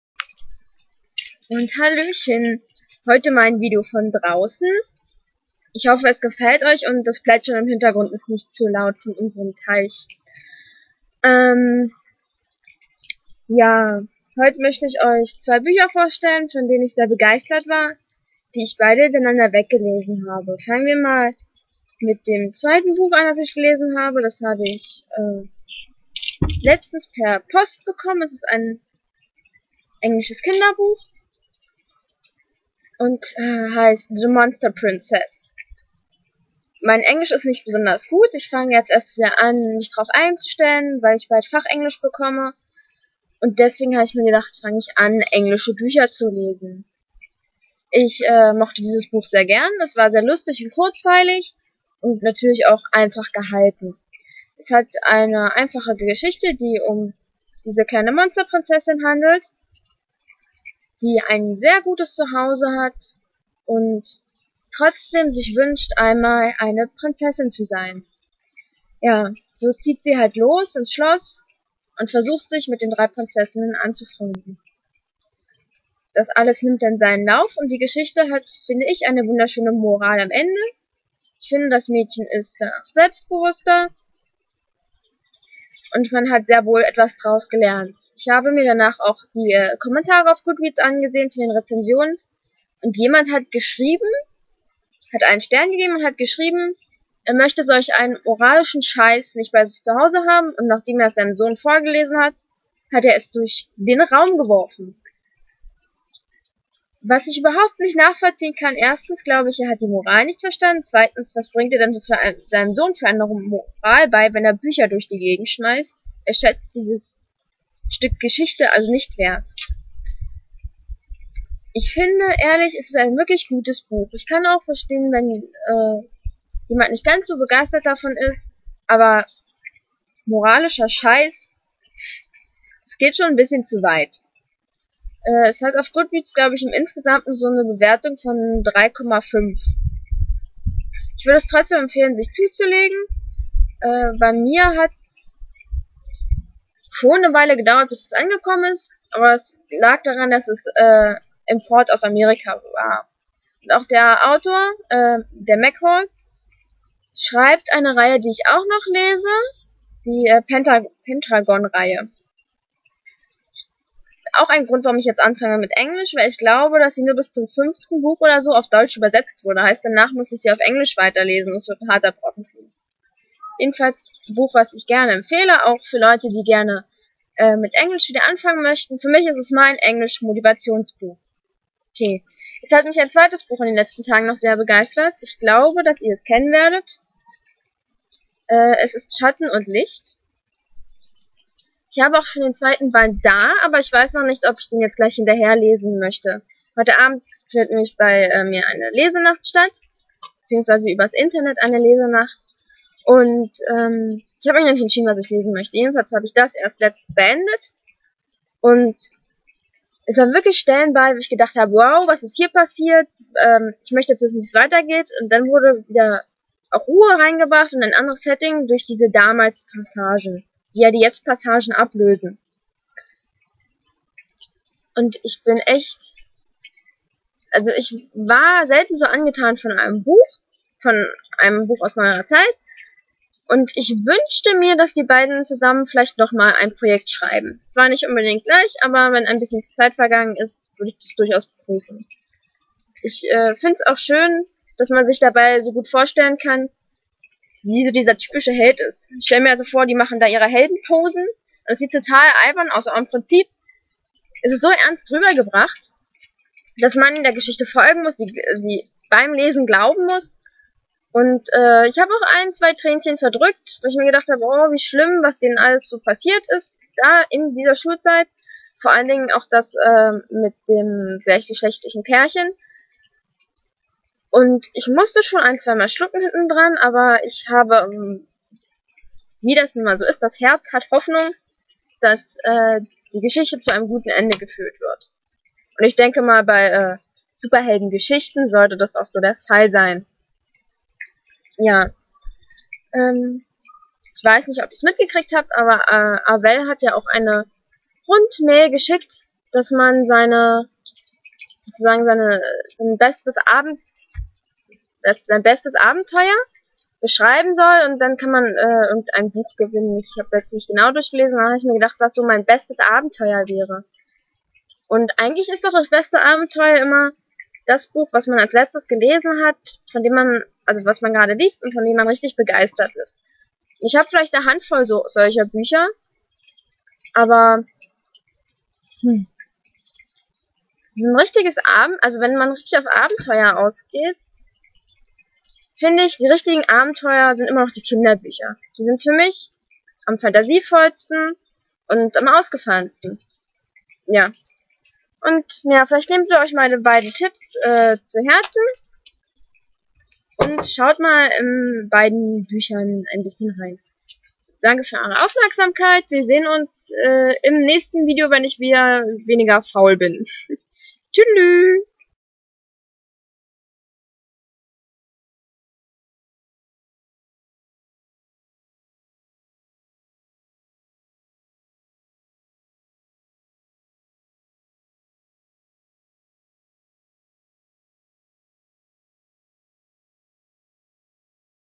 [Empfehlungen] (Super)Helden groß und klein!
▼Klappentext▼

Ich empfehle sehr selten Bücher, deshalb ich das hier schon eine Besonderheit.

"Das Ikarus-Projekt - Schatten und Licht Bd 1"
by Jackie Kessler und Catlin Kittredge (Autorinnen)

"The Monster Princess"
by D.J. MacHale (Autor)
 Alexandra Boiger (Illustrator)
▼Anlagen▼

Text und Stimme von KillingRomance.
Genutze Programme:
OpenOffice, Audacity, Gimp 2, Picasa, WindowsMovieMaker